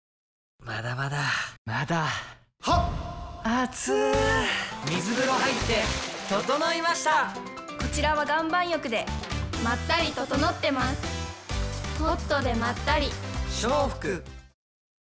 祥福 サウナ篇 ２０２０年８月
祥福CM 15秒サウナバージョン

出演：
吉田 涼哉　(BREAK THROUGH)
伊藤 純平　(BREAK THROUGH)
寺浦 麻貴
滝川 楓